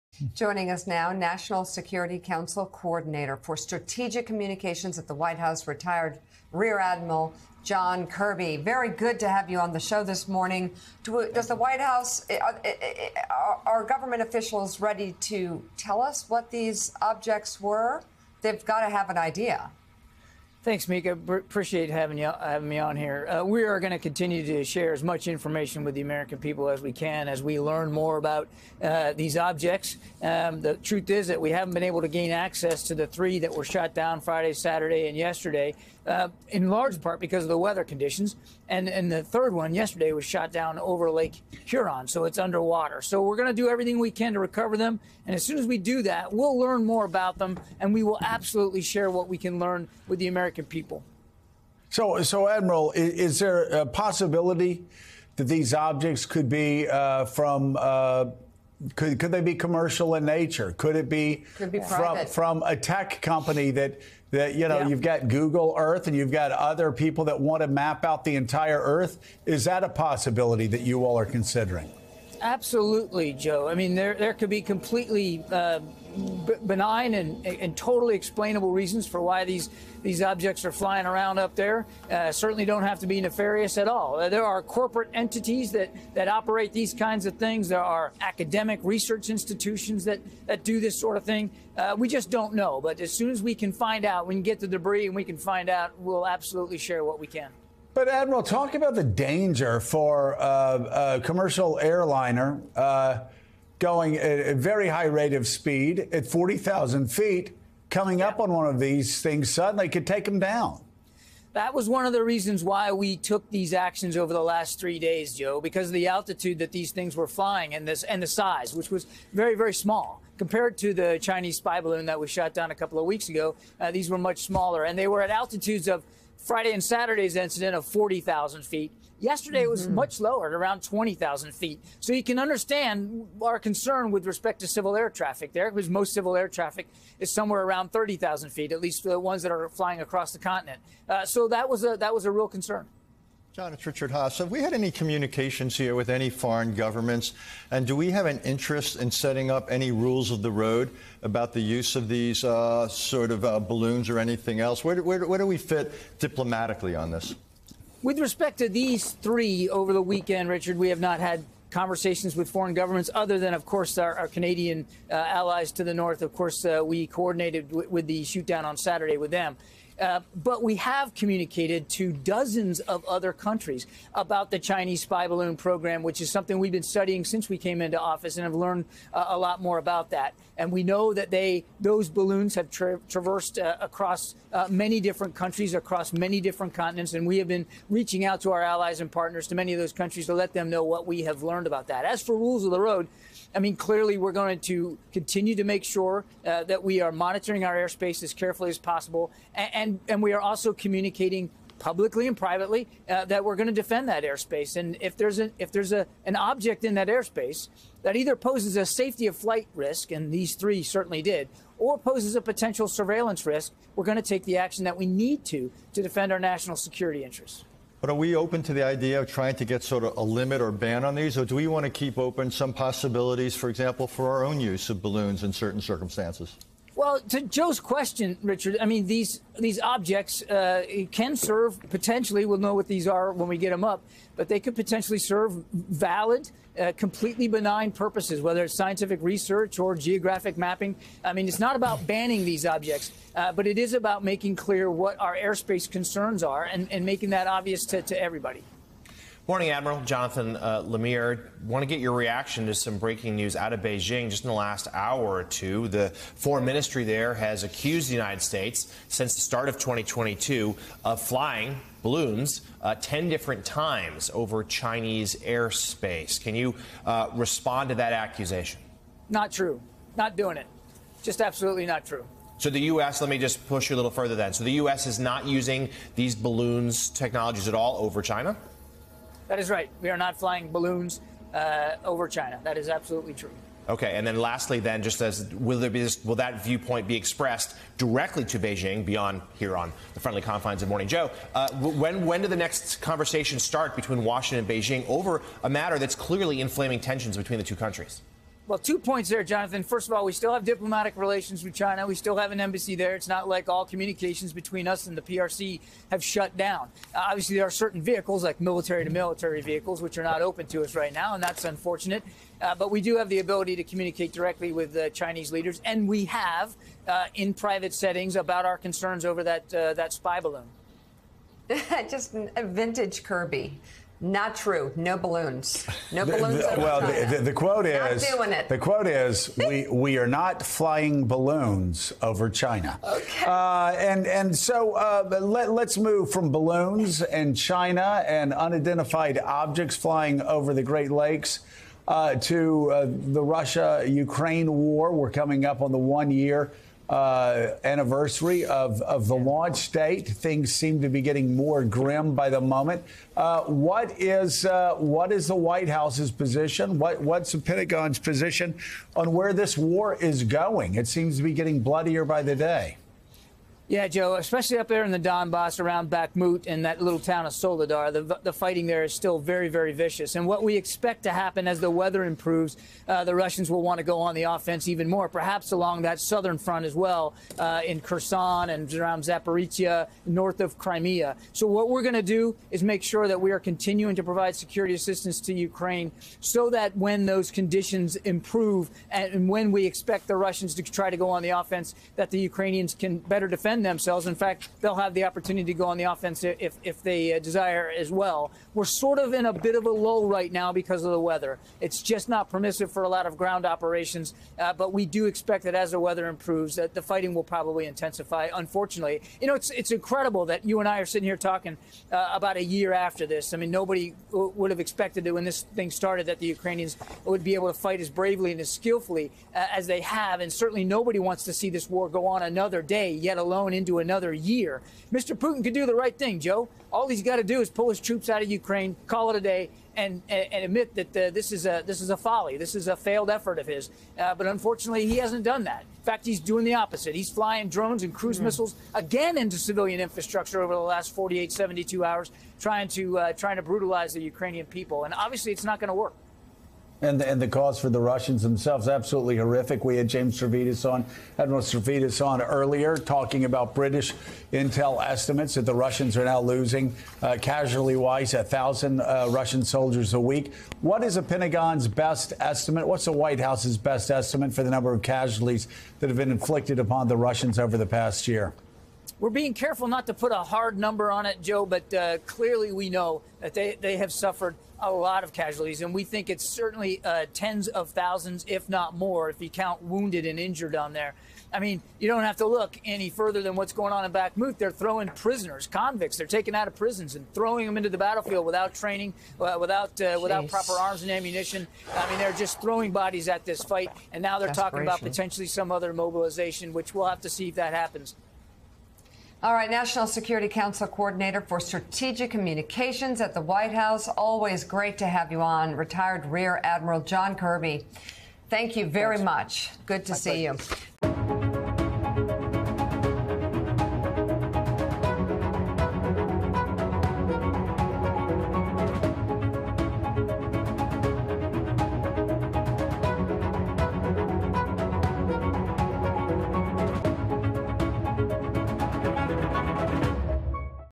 John Kirby: Domestic air travel a concern in downing of flying objects
National Security Council coordinator for strategic communications at the White House, John Kirby, discusses the downing of three more unidentified flying objects. Kirby says the objects don't have to be 'nefarious at all'.